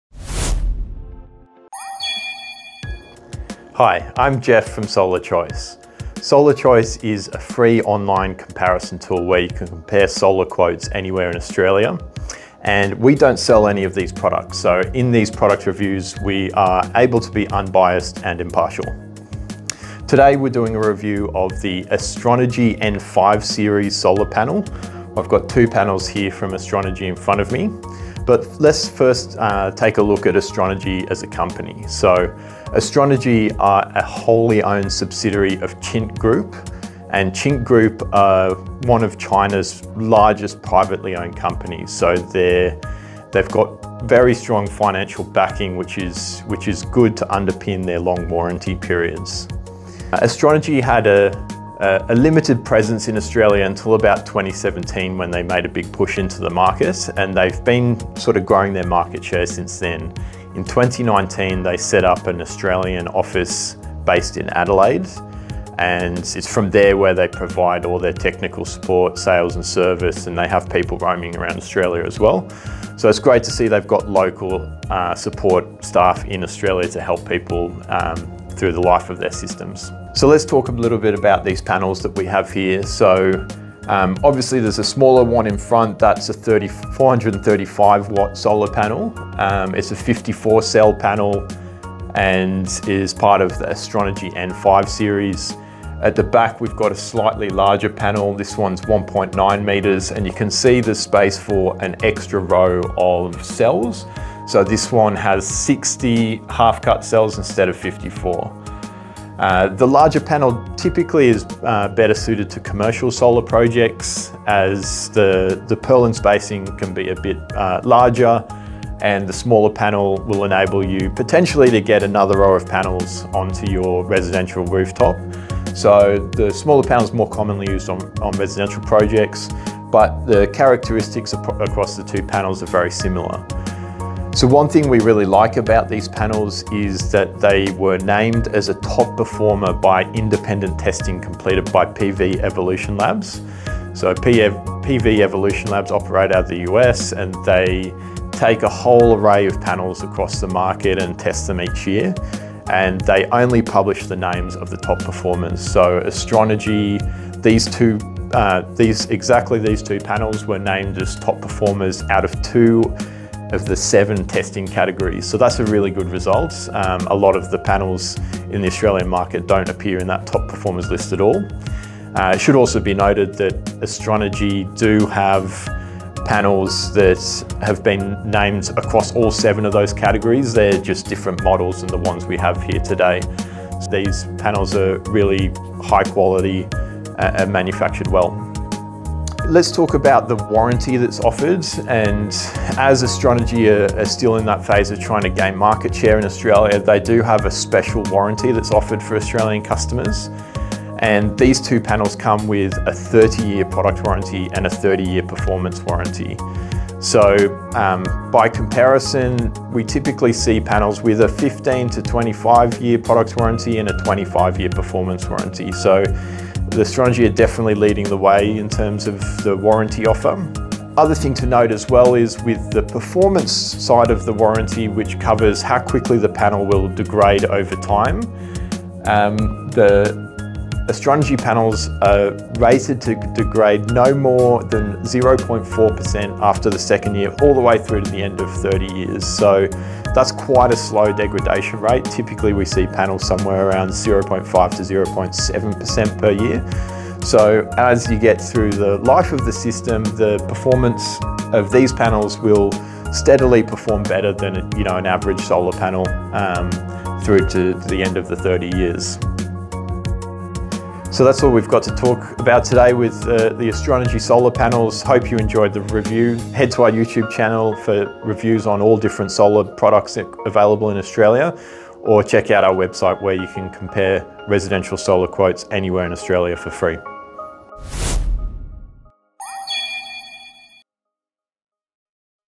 Astronergy Solar Panel Review: Are They Worth It? An Independent Guide
Explore the pros, cons, performance, & reliability of Astronergy solar panels in this detailed review. Discover why these panels are recognized as top performers by PV Evolution Labs and learn about their warranty offers exclusive to Australian customers.

Timestamps:
0:03 - Introduction with Jeff from Solar Choice
0:21 - Overview of Astronergy Solar Panels
1:24 - Detailed Look at 435 Watt Panel Features
2:01 - Benefits of Smaller Residential Panels
2:17 - Test Results from PV Evolution Labs
3:10 - Unpacking the 30-Year Warranty Details
3:46 - Degradation Rates and Long-term Performance
4:24 - Conclusion: Are Astronergy Panels Right for You?

🛈 What We Cover:

Performance: Insights into the 435-watt panel specifications and what makes these panels stand out.
Local Support: The benefits of having local support staff in Australia for Astronergy products.
Quality Assurance: How Astronergy panels perform in independent tests and their placement in the top performer's category.
Warranty: Details of the 30-year product and performance warranty for Australian customers.
Longevity: Discussion on the degradation rate of Astronergy panels compared to other brands.